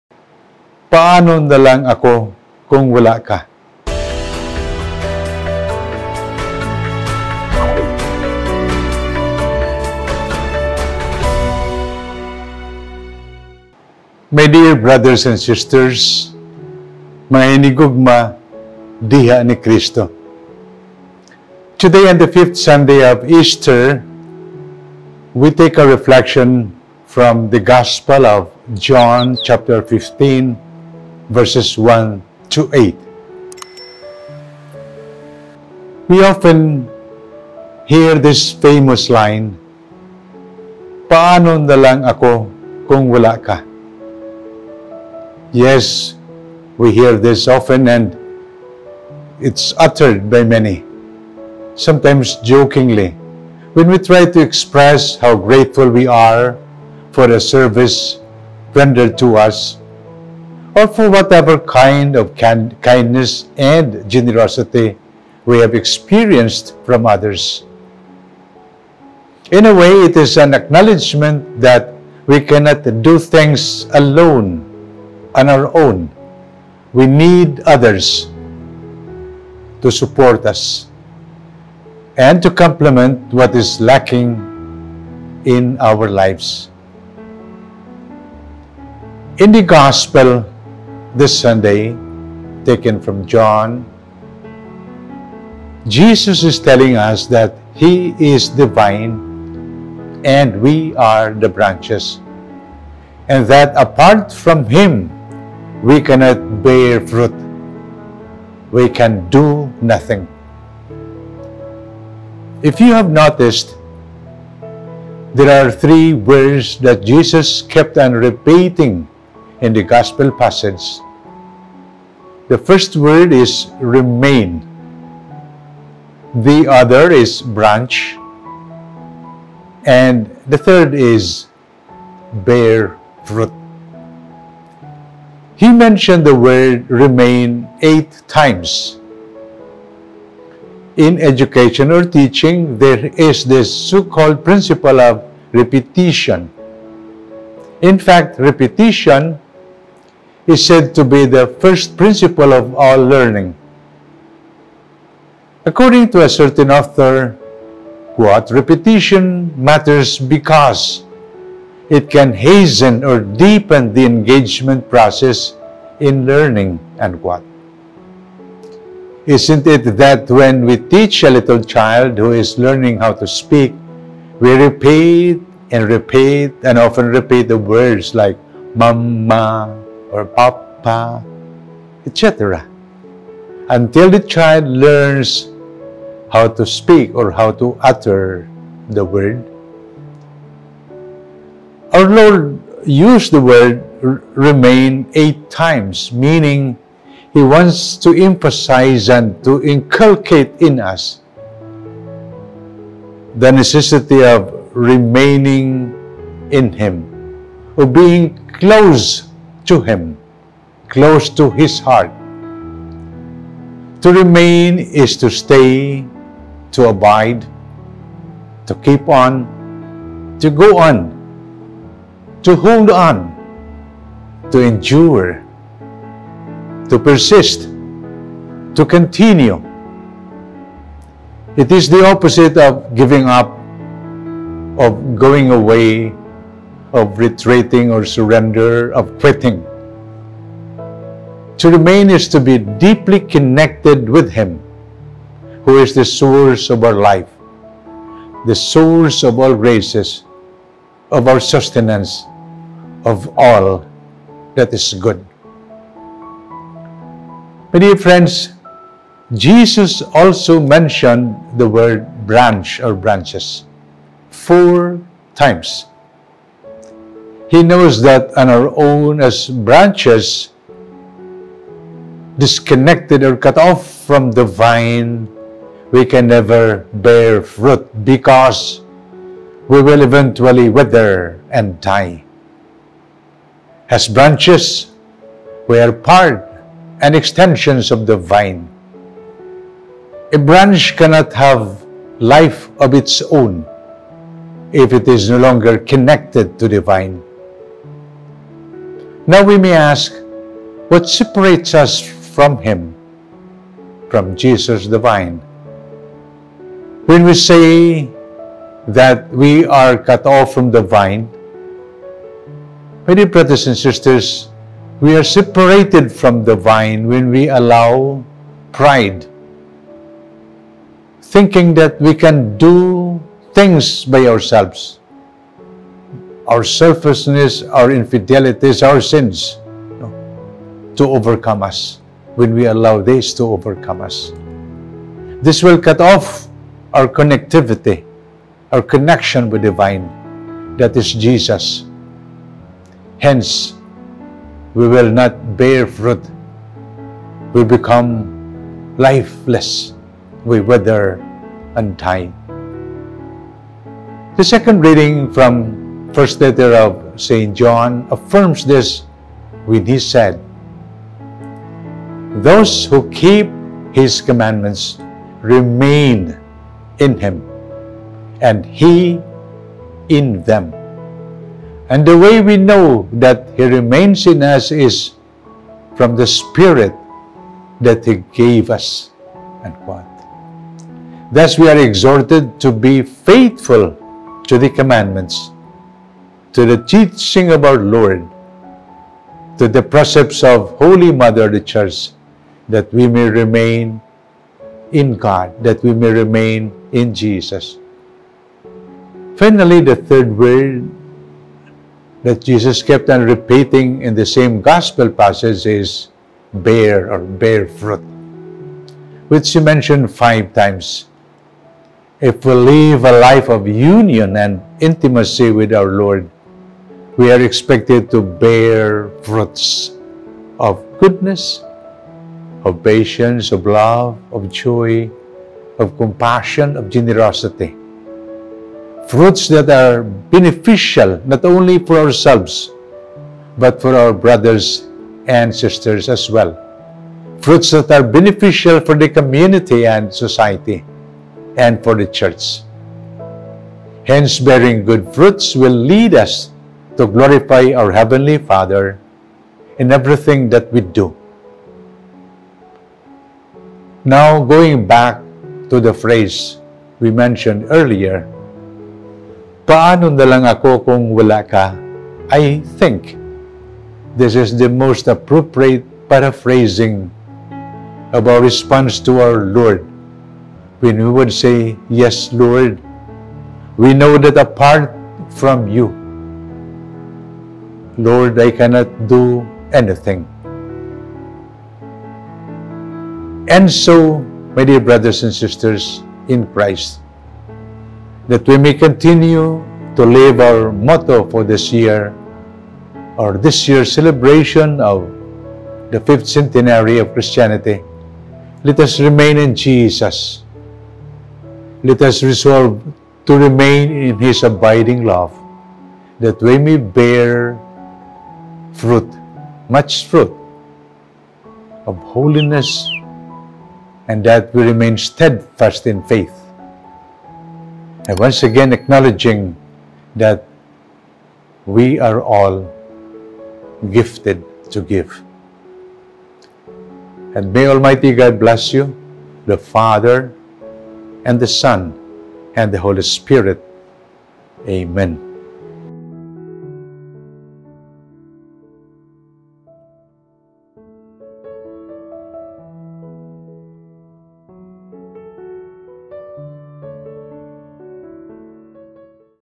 PAANO NA LANG AKO KUNG WALA KA? | Reflections 2021: 5th Sunday of Easter
PAANO NA LANG AKO KUNG WALA KA? |  Cebu Archbishop Jose S. Palma reflects on the importance of remaining in God always.